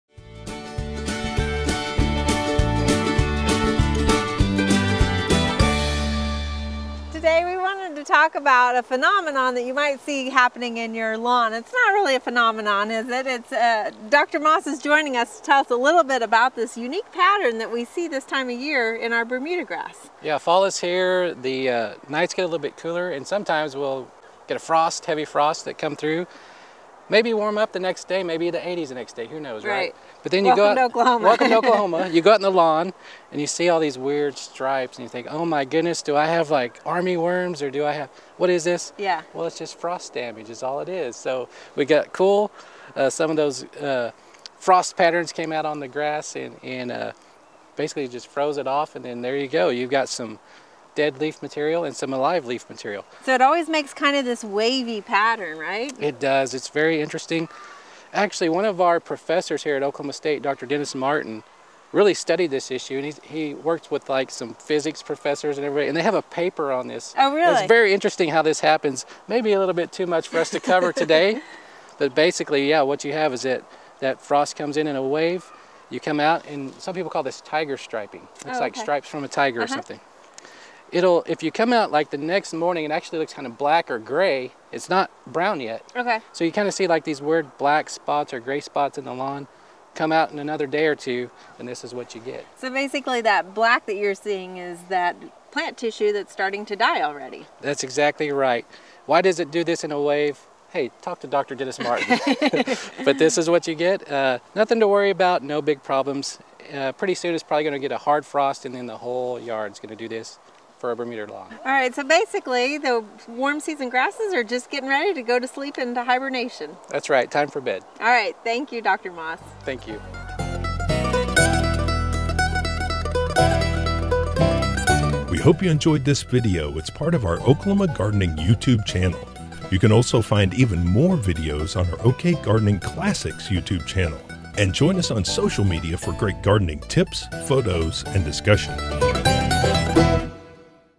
Tiger Stripes in your lawn
At the Cimarron Valley Research Station in Perkins, OK, OSU Turf Grass Specialist, Justin Moss, shows us the start of a new Bermudagrass removal study.

Questions?